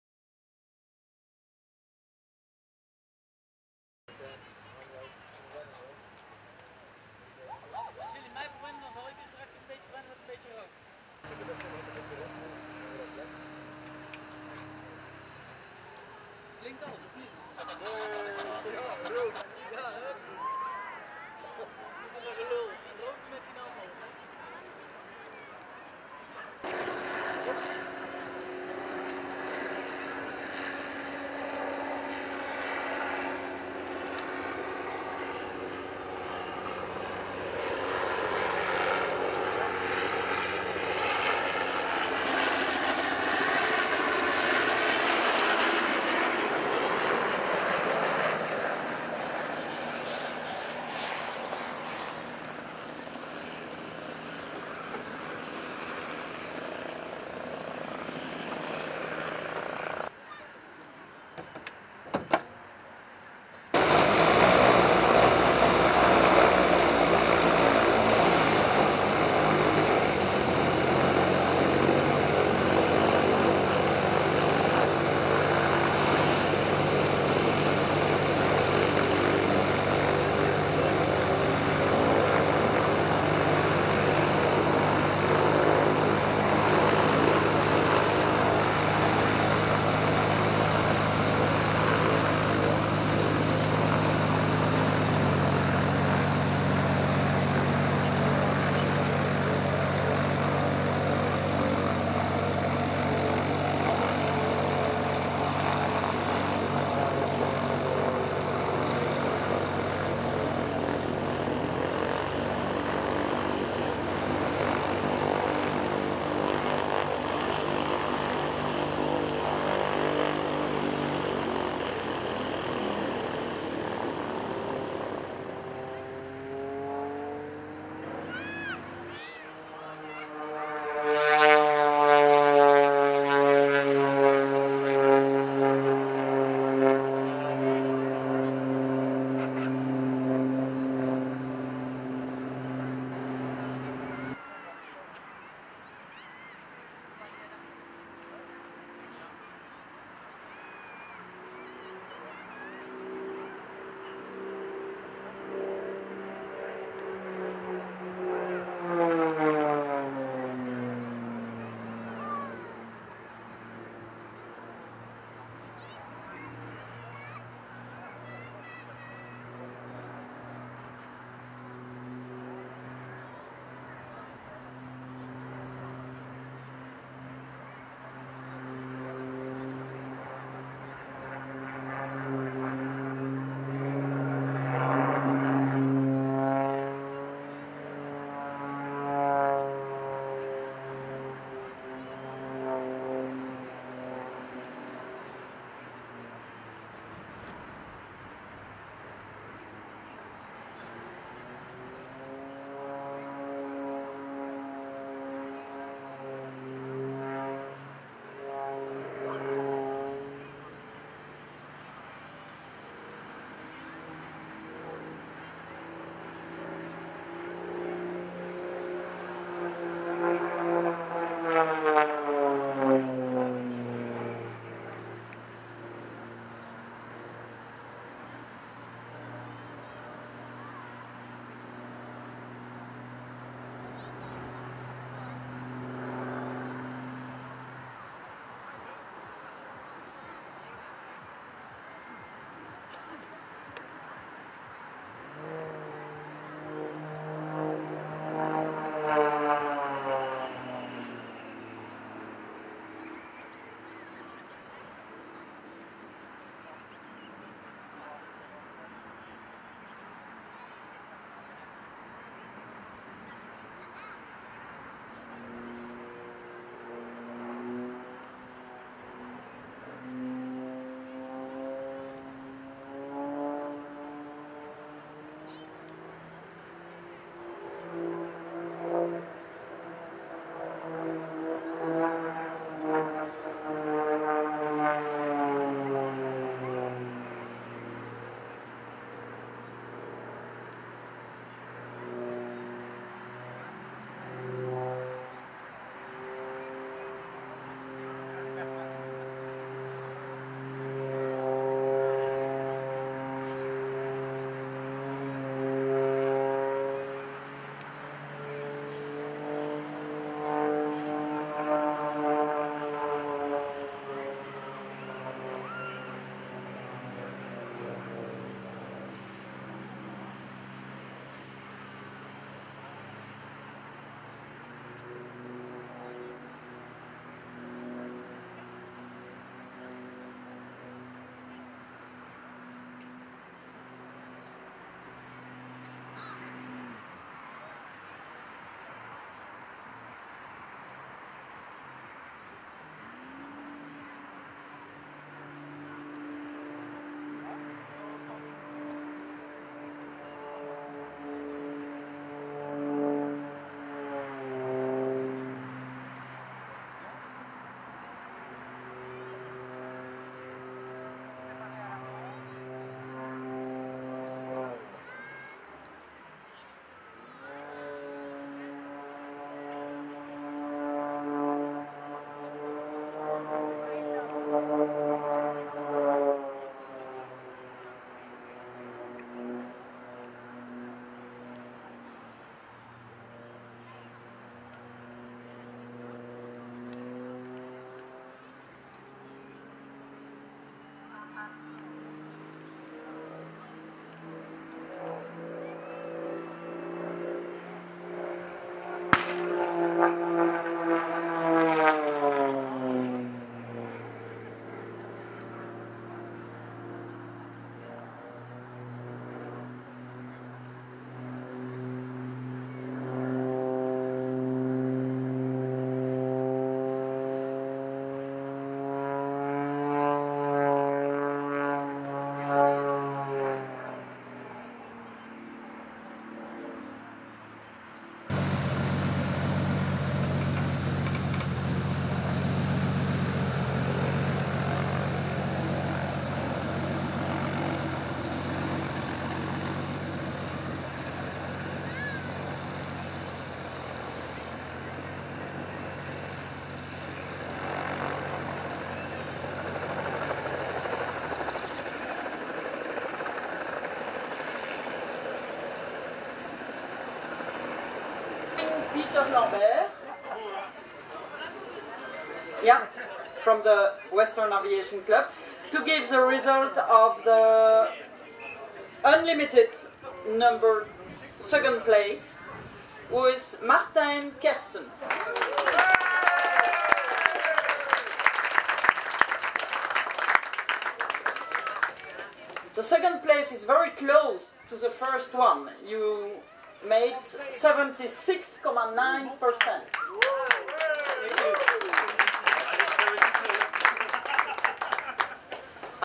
4K UHD Martijn Kersten Extra 330 Sc Vice Belgian Champion unlimited Free Style BOAC 2022 Koksijde
Martijn Kersten Extra 330 Sc   Vice Belgian Aerobatic Champion  unlimited Free Style BOAC 2022 Koksijde. 2 & 3/07/2022.

Weather : Partly cloudy 20° C